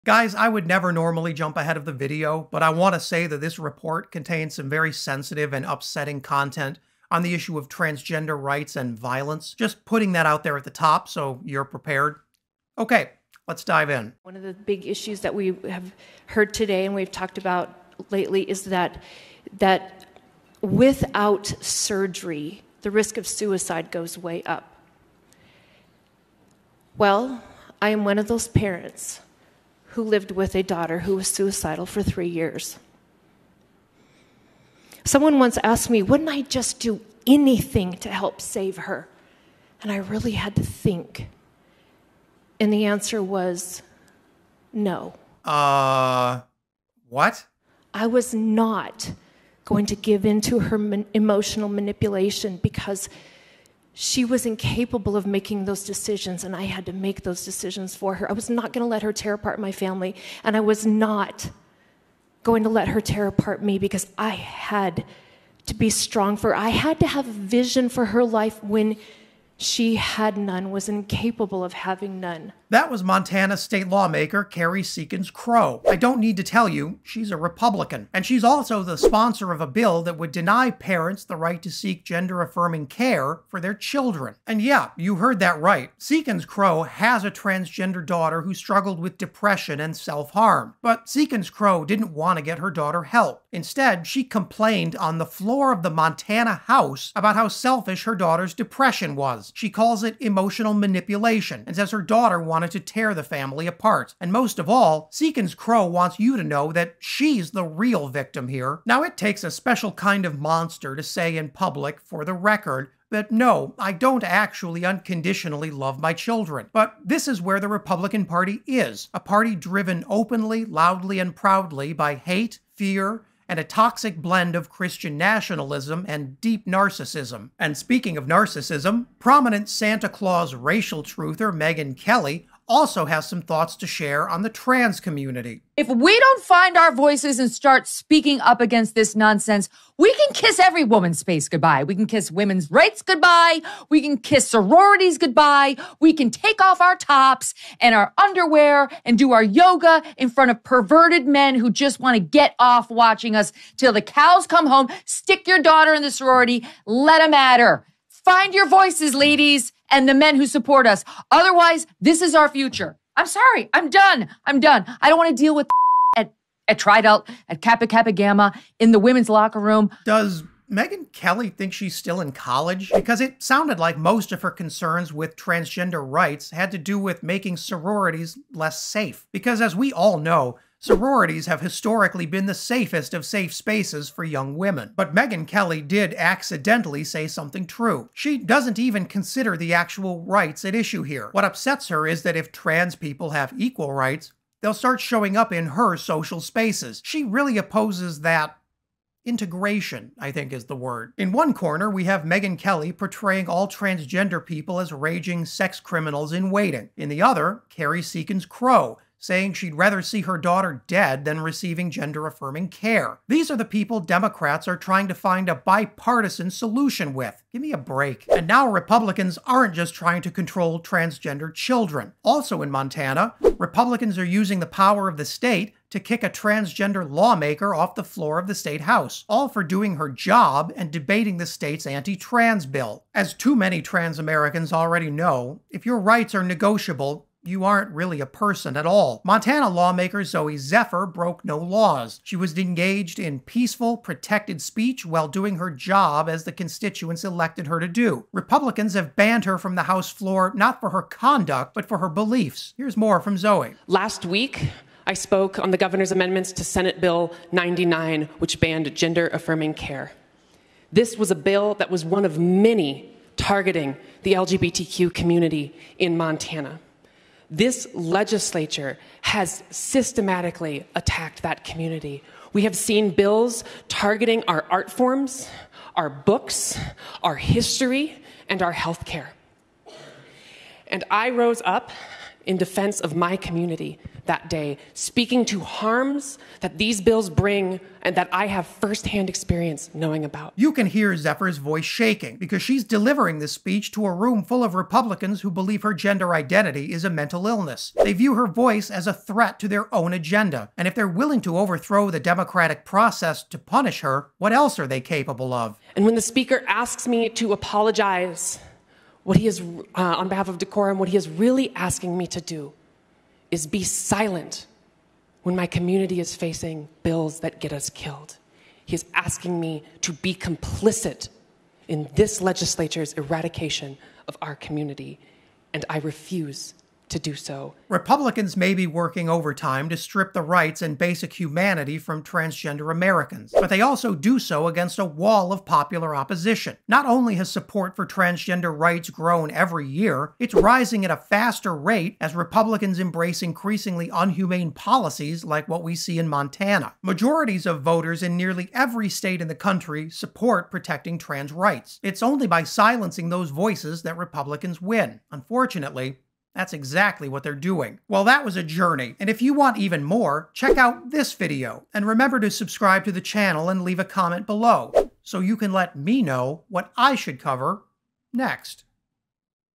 MT Republican Says The Unimaginable About Her OWN Daughter
Montana State Representative Kerri Seekins-Crowe made it clear that she would rather risk her own child's life than support their gender transition. Seekins-Crowe's remarks are a clear example of the intolerance and bigotry that continues to plague our society, and serve as a reminder that we still have much work to do in order to achieve true equality and justice for all. Max Burns breaks it down.